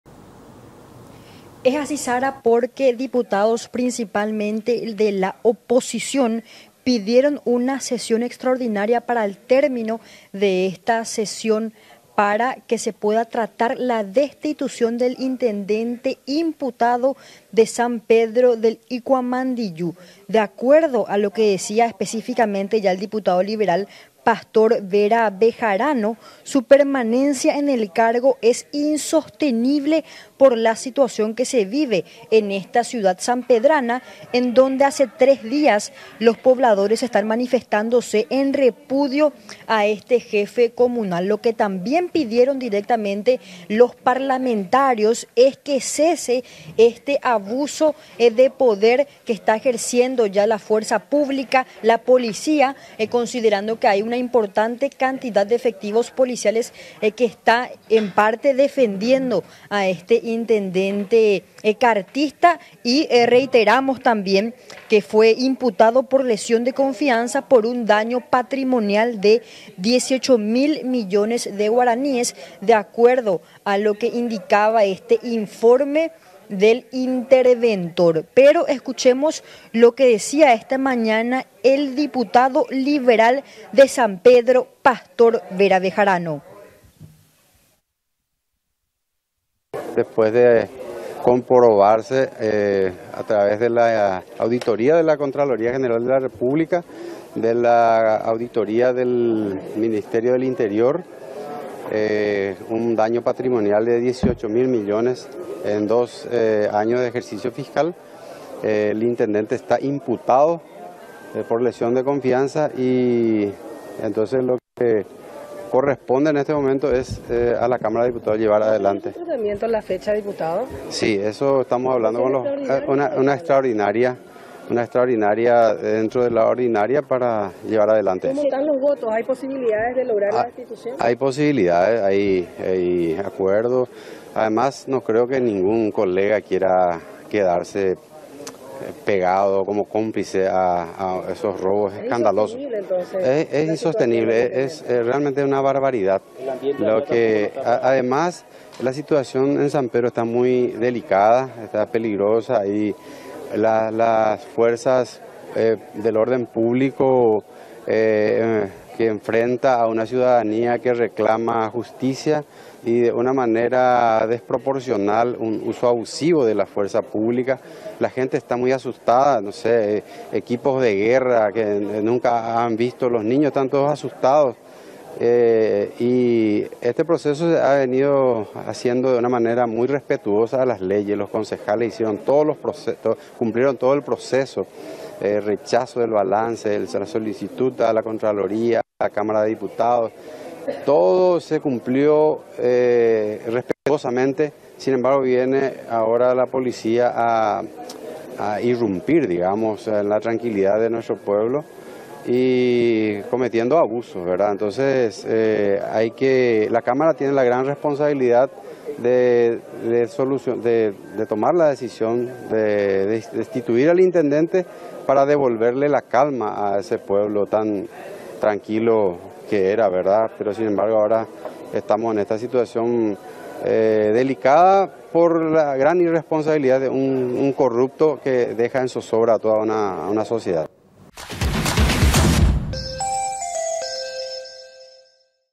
Diputados urgen destitución de intendente
El diputado liberal Pastor Vera Bejarano adelantó que presentará este miércoles un pedido de sesión extraordinaria para tratar la destitución del intendente colorado cartista de San Pedro del Ycuamandyyú, Gustavo Rodríguez. El jefe comunal fue imputado por lesión de confianza por un daño patrimonial de 18.000 millones de guaraníes. Para el parlamentario, su permanencia en el cargo es “insostenible” por la situación que se vive en la ciudad sampedrana.